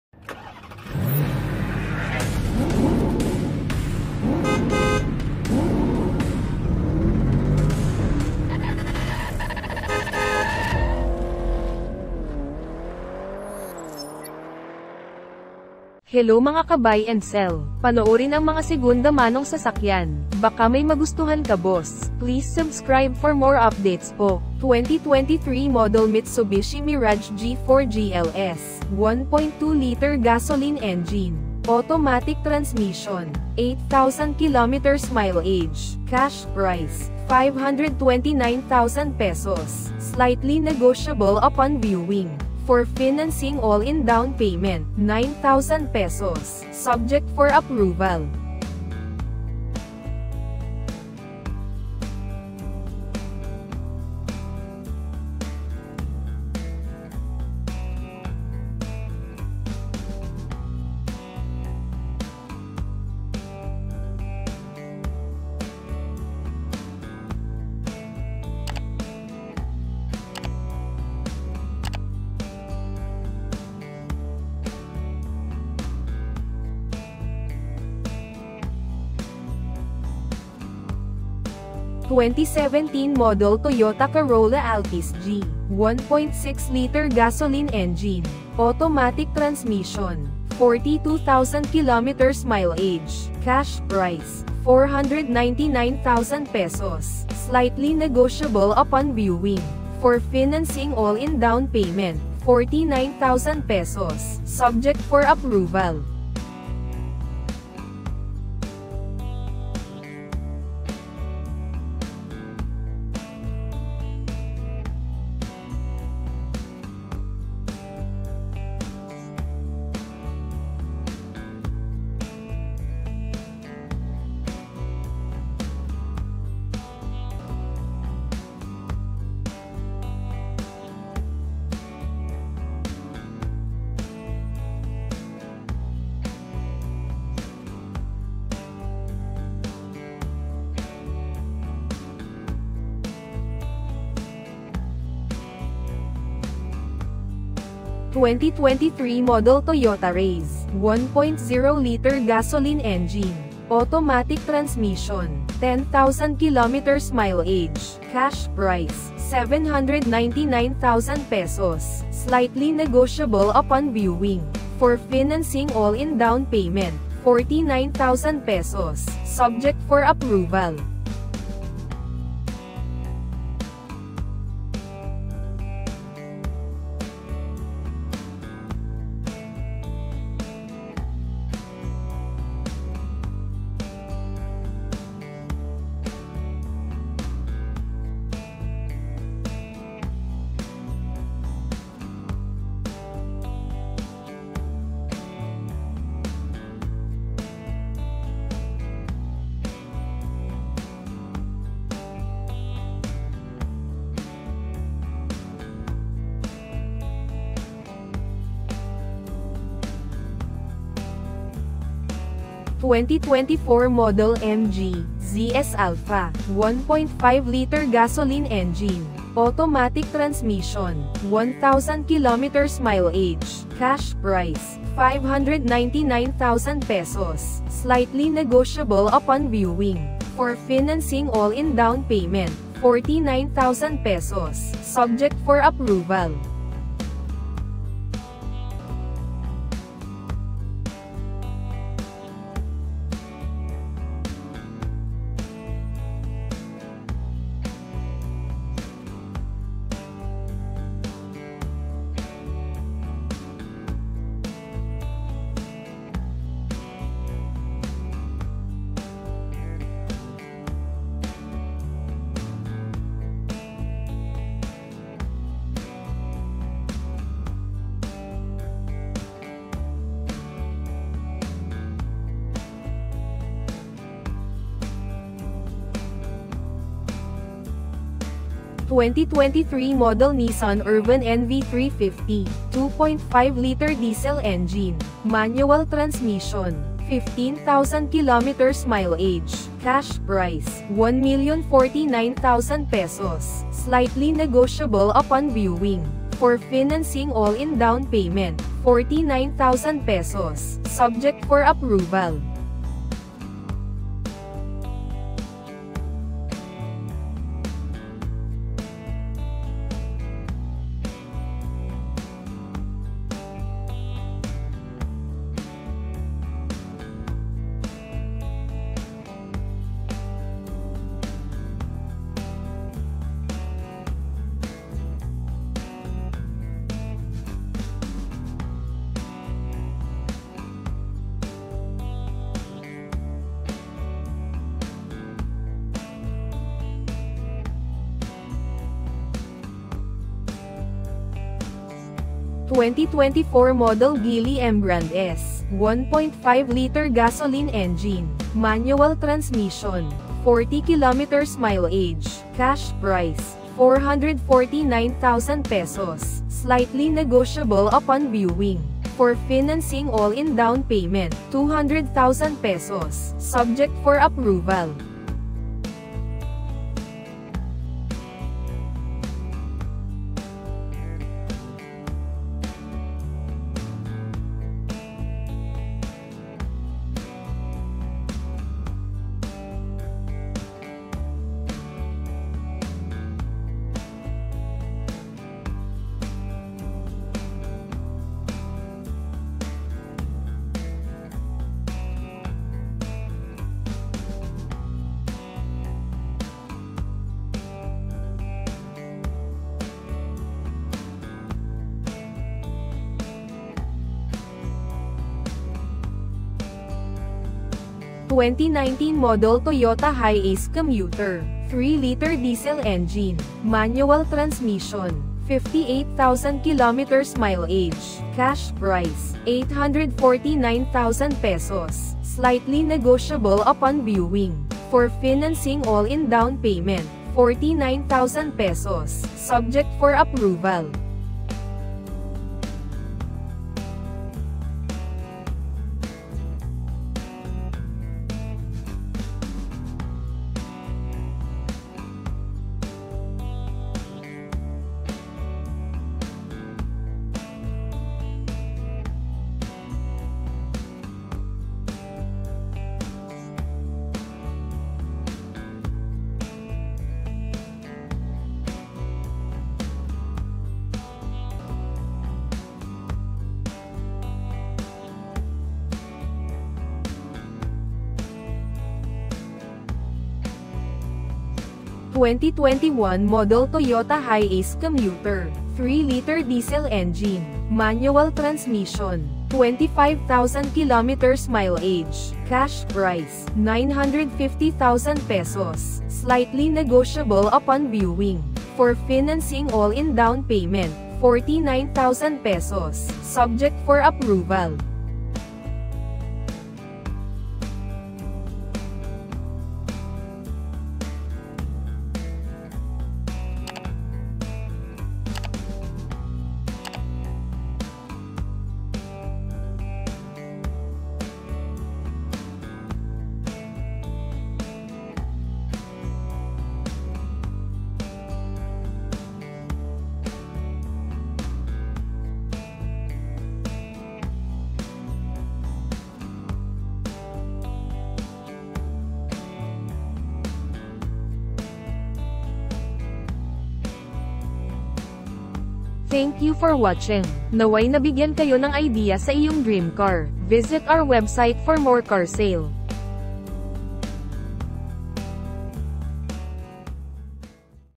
December 2024's HOTTEST Used Car Deals in the Philippines!
Second Hand Quality Used Cars in Philippines | List of Pre-owned Car for Sale in Basmarket

"Trust in the Lord with all your heart, and lean not on your own understanding: In all your ways acknowledge Him, and He shall direct your paths."

👉 For more details of pre-owned cars listing above, Visit here:

2023 Mitsubishi Mirage G4 GLS 1.2L Gas Automatic
1.2-liter Gasoline Engine
Automatic Transmission
8,000km Mileage

2017 Toyota Corolla Altis 1.6 G Automatic Gas
1.6-liter Gasoline Engine
Automatic Transmission
42,000km Mileage

2023 Toyota Raize 1.0L Turbo Gasoline Automatic
Top of the Line!
1.0-liter Turbo Gasoline Engine
Automatic Transmission
10,000km Mileage

2024 MG ZS Alpha 1.5L Gasoline Automatic
1.5-liter Gasoline Engine
Automatic Transmission
1,000km Mileage

2023 Nissan Urvan NV350 15-Seater Manual Diesel
2.5-liter Diesel Engine
Manual Transmission
15,000km Mileage

2024 Geely Emgrand 1.5 S Manual Gasoline
1.5-liter Gasoline Engine
Manual Transmission
40km Mileage

2019 Toyota Hiace Commuter 3.0L Manual Diesel
3.0-liter Turbo Diesel Engine
Manual Transmission
58,000km Mileage

2021 Toyota Hiace Commuter 3.0L Manual Diesel
3.0-liter Turbo Diesel Engine
Manual Transmission
25,000km Mileage